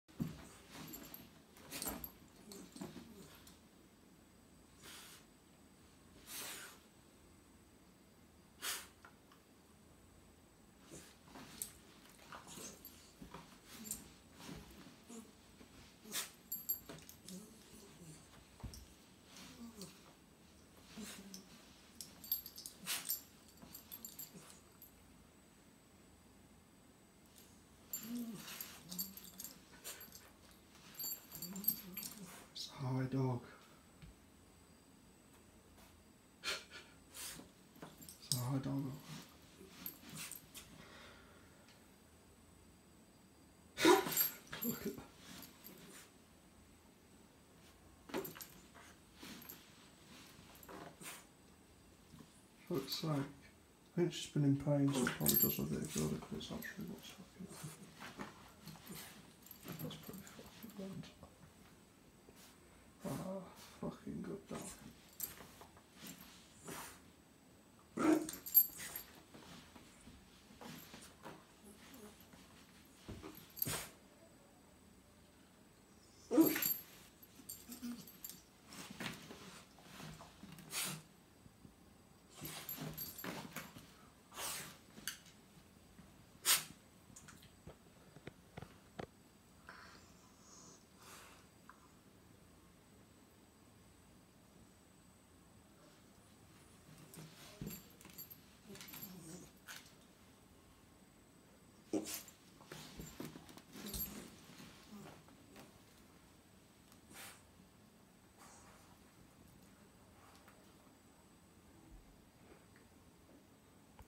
Tibetan Terrier High On Cannabis
Second hand kief smoke